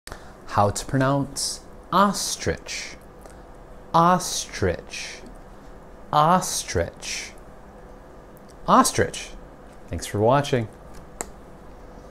How to Pronounce Ostrich in English
In this video we show the correct way to say ostrich in American English.